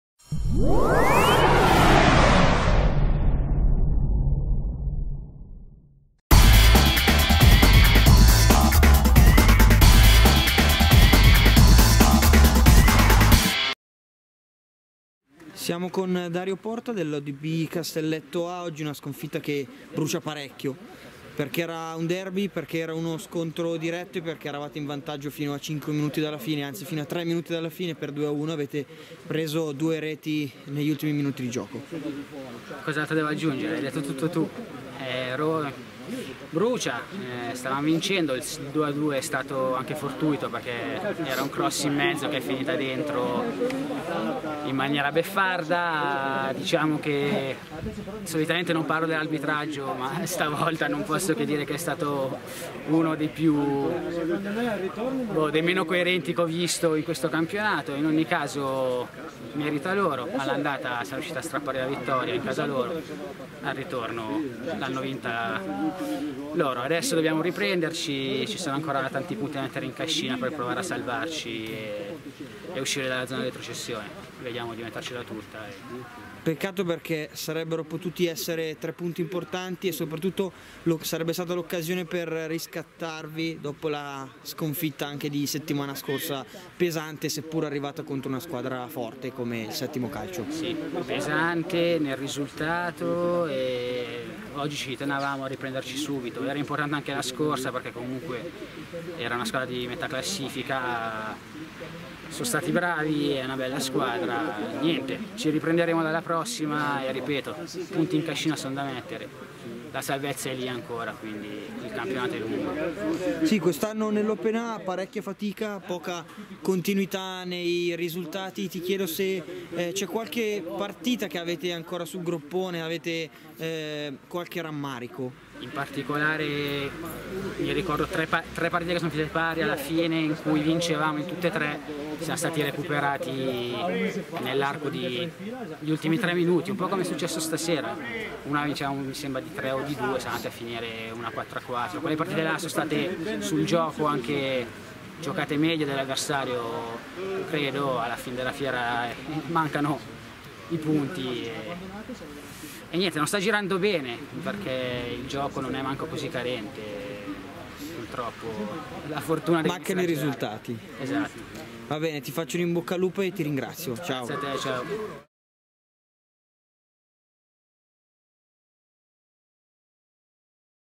Intervista Odb Castelletto A - Dario Porta
Le parole del giocatore dell'Odb Castelletto A dopo la sfida contro Polis Senago.